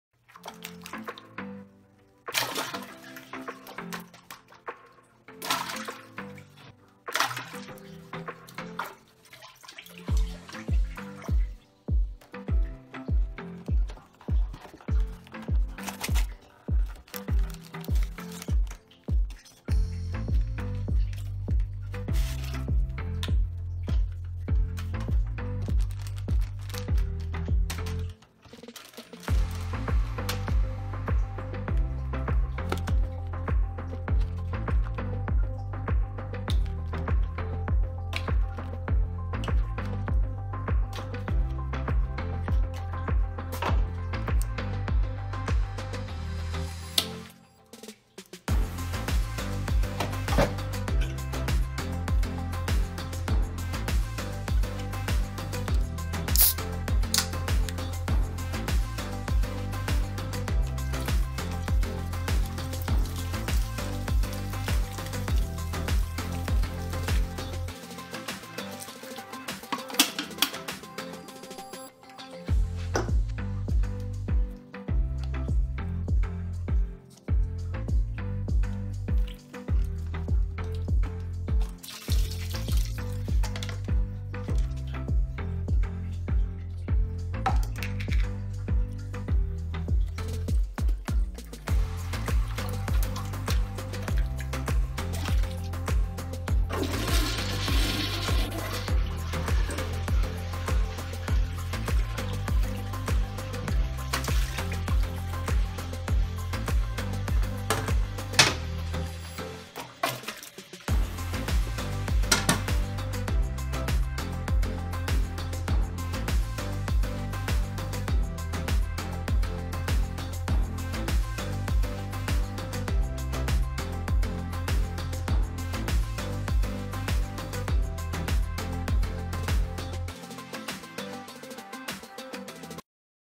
Will it Flush? - McDonald's Happy Meal, Coca Cola, Fanta, and Mirinda Balloons
Don't try this experiment yourself!